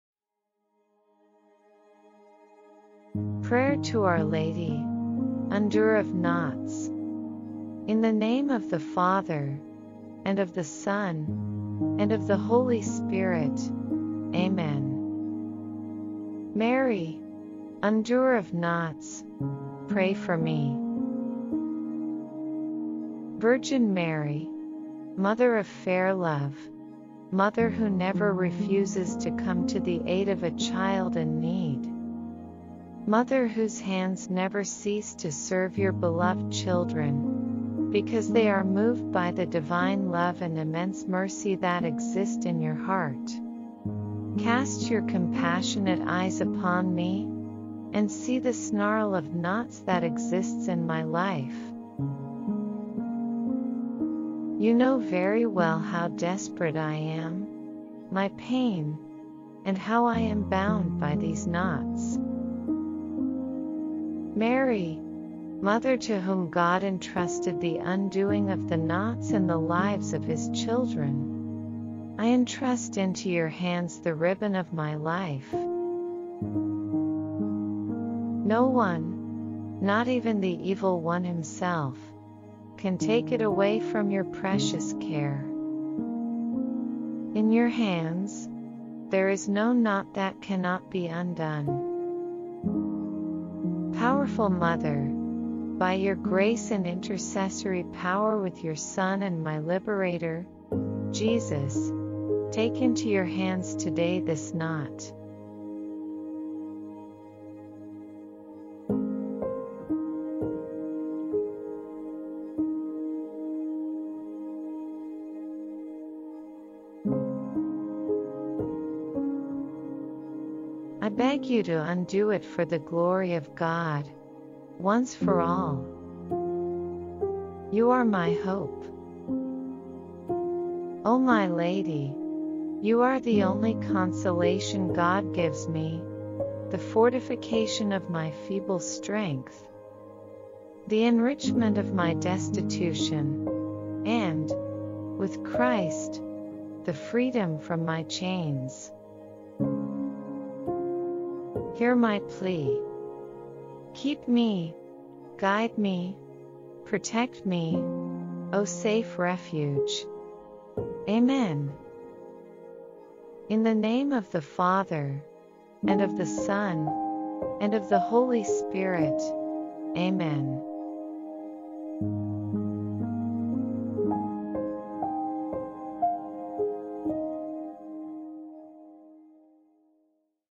Prayer to Our Lady, Undoer of Knots
Mary, referred to as the Untier of Knots or Undoer of Knots, is a significant figure in both a revered Marian devotion and a remarkable Baroque artwork known as a "Wallfahrtsbild" or "Gnadenbild."

This particular painting, created by Johann Georg Melchior Schmidtner around 1700, finds its home in the Catholic pilgrimage church of St. Peter am Perlach, also known as the Perlach church, located in Augsburg, Bavaria, Germany.

Initially, devotion to this image was predominantly observed in specific Latin American countries like Argentina and Brazil. However, it gained worldwide recognition and popularity following the election of Pope Francis.

The feast day of Our Lady, Undoer of Knots, is celebrated on September 28th primarily because it coincides with a significant event in the Catholic Church calendar. September 8th is the feast of the Nativity of the Blessed Virgin Mary, and the feast of Our Lady, Undoer of Knots, is observed exactly 20 days later, on September 28th.

Our Lady, Undoer of Knots, pray for us.

Special thanks to: @DeepInstrumental